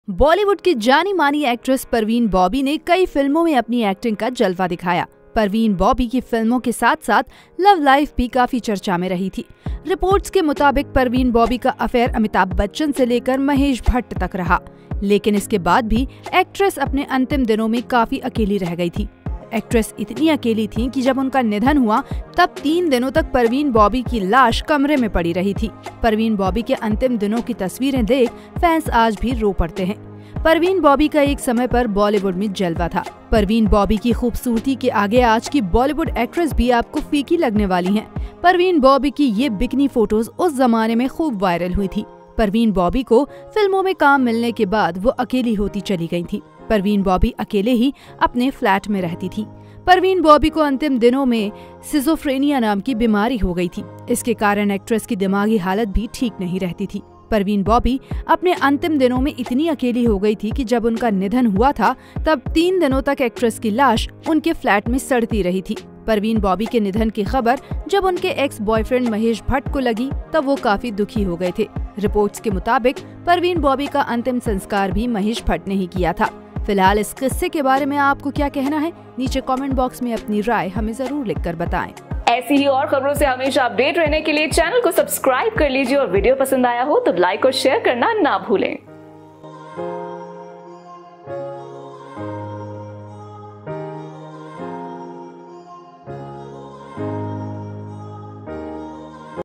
FULL STORY : Parveen Babi की 3 दिन तक फ्लैट में सड़ती लाश... Ex-BF ने करवाया अंतिम संस्कार
Parveen Babi De@th Anniversary Her Tragic Last Days Postmortem Report Cause Of De@th And How Property Was Divided | Parveen Babi Mahesh Bhatt Love Story De@th Reason And Funeral Controversy

EDITOR :--Shoaib